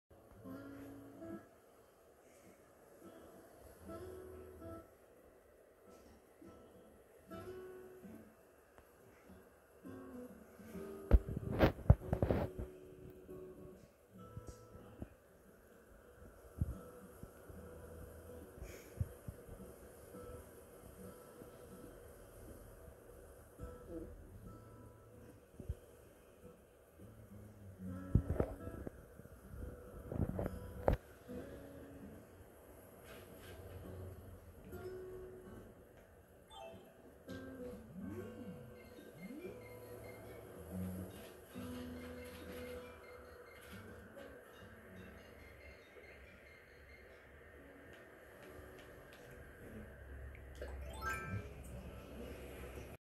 Cat licks dog
A video I took of my pets during the first day of my quarantine.